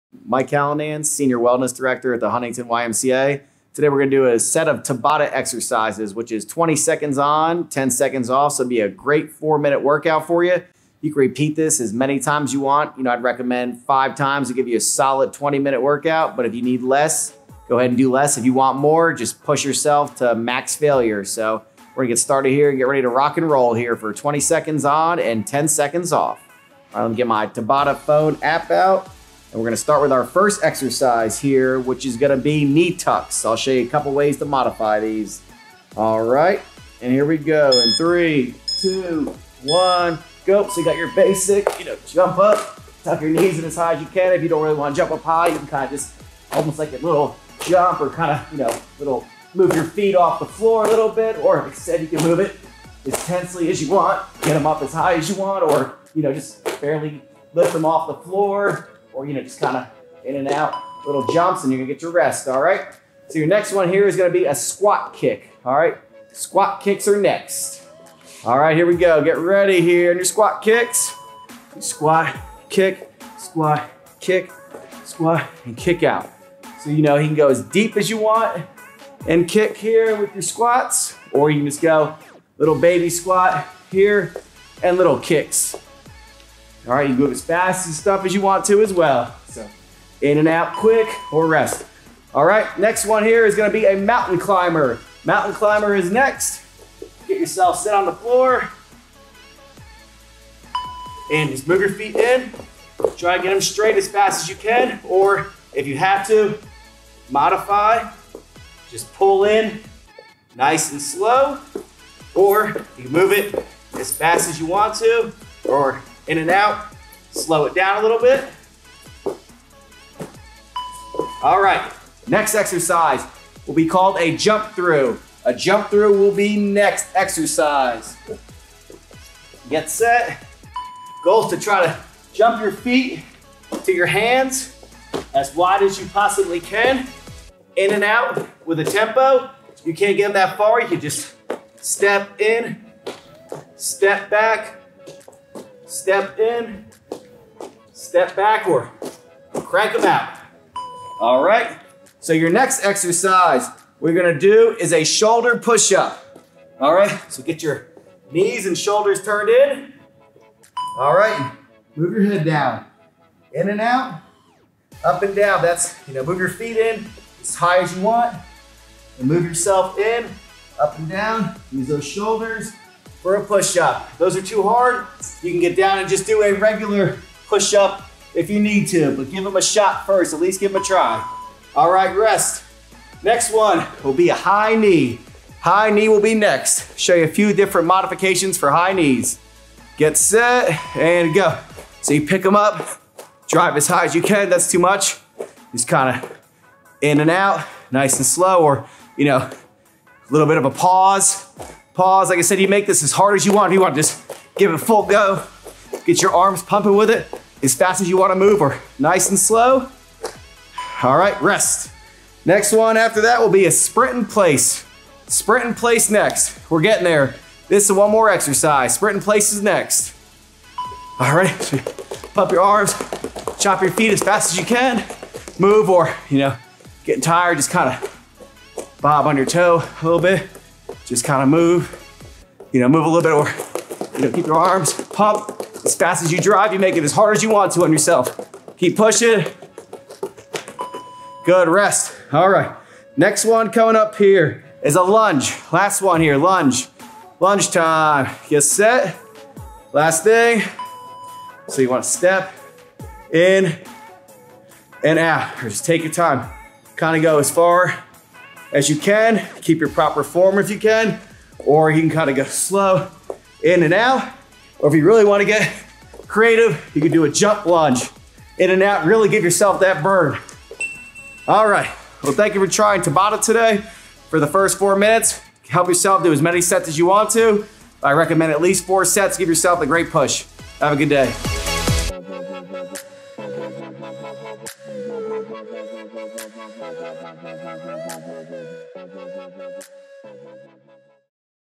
Intense Tabata 4-Minute Challenge
Mike Callanan, Senior Health & Wellness Director, has got an intense Tabata workout, but don't worry -- there are plenty of ways to modify the exercises to work for anyone.